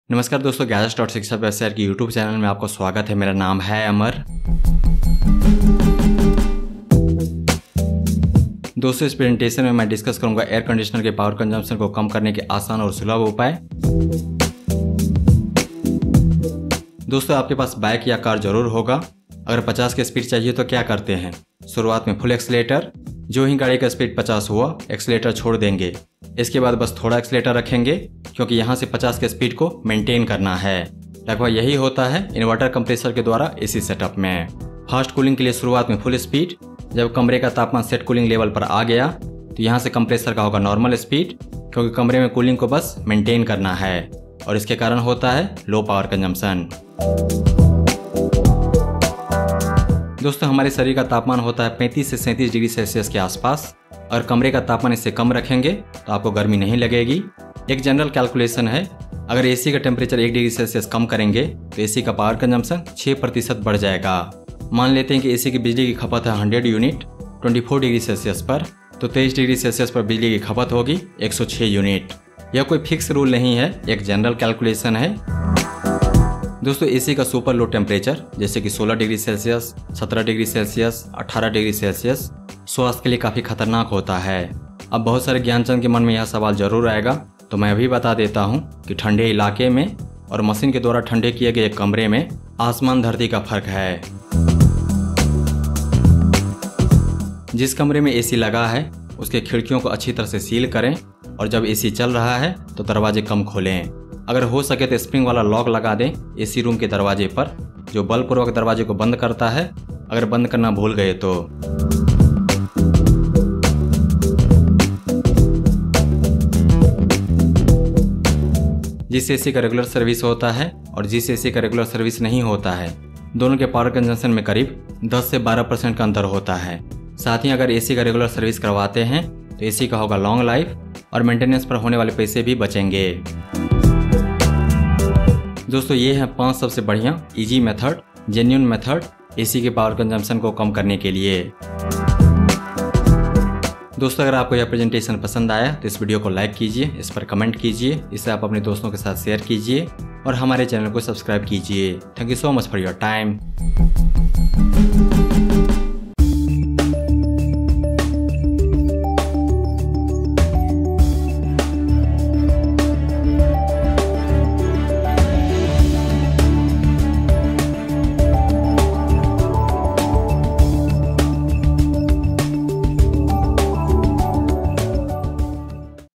🔥5 Easy Tips to Reduce AC Power Consumption {हिंदी में} ► Air Conditioner Energy Saving Methods
This presentation covers five simple and easy methods to reduce ac power consumption. By following those tips & tricks will help in reducing the power consumption of your air conditioner by up to 50%.